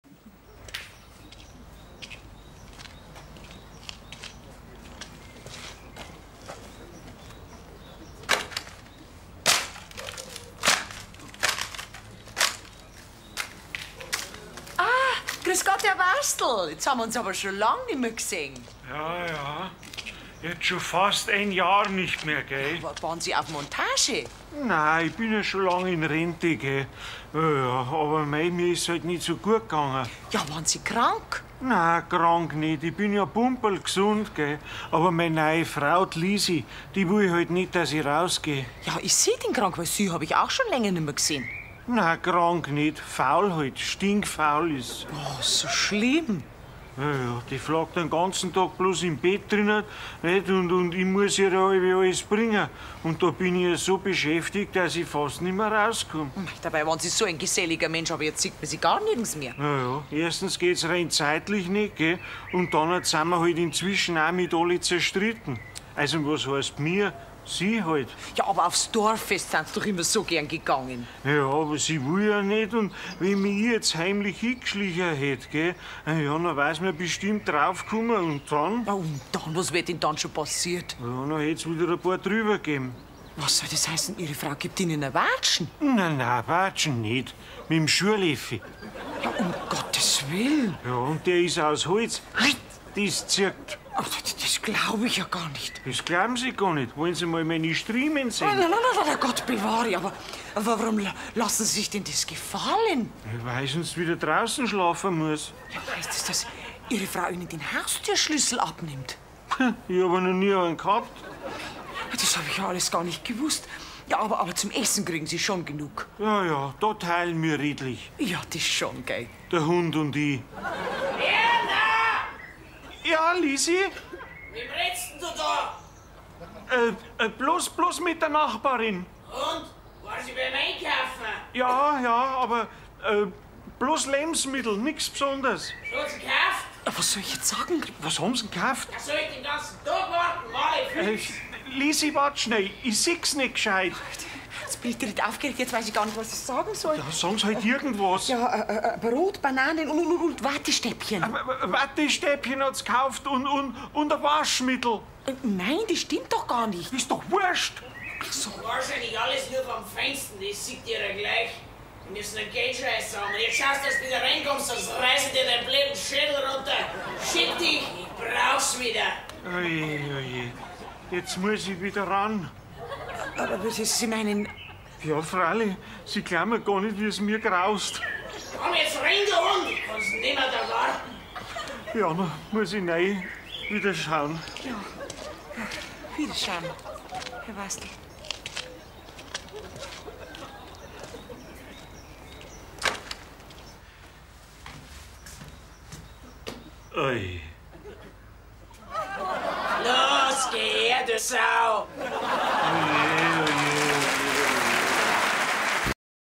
Der arme Herr Wastl
Günther Grünwald und Monika Gruber in einem Nachbarschafts-Sketsch